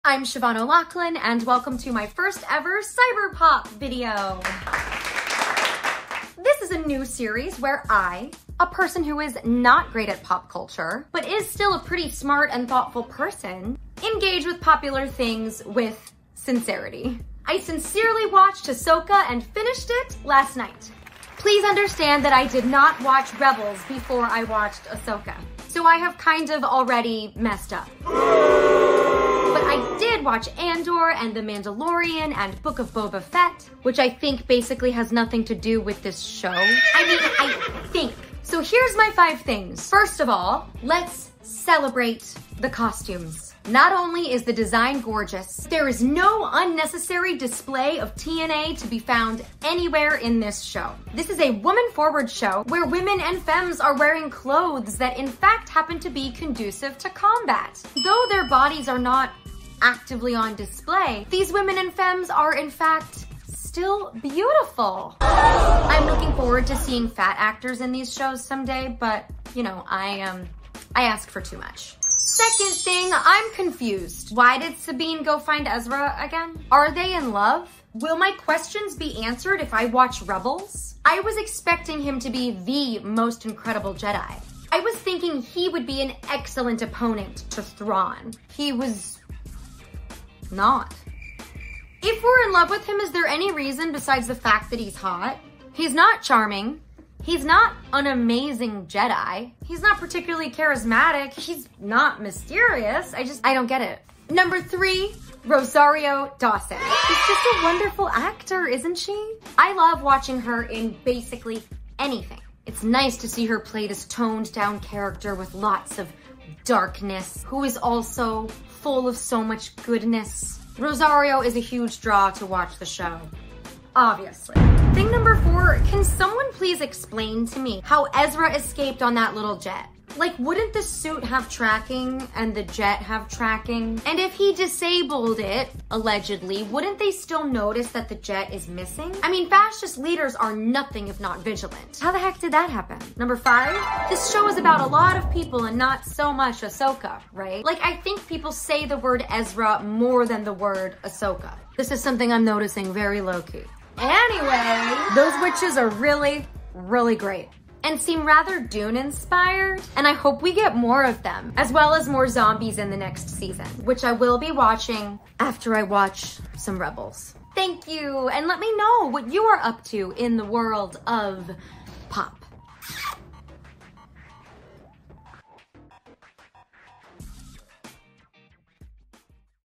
Cyber Pop Episode One: AHSOKA
Welcome to my brand new series, where I engage in any type of pop culture -- tv, film, music, music VIDEOS, books, graphic novels -- all of the above, and share my thoughts and feels about it. Why? Because you asked for it.

My first episode is my review of the series AHSOKA.

Music here is by Ben Sound.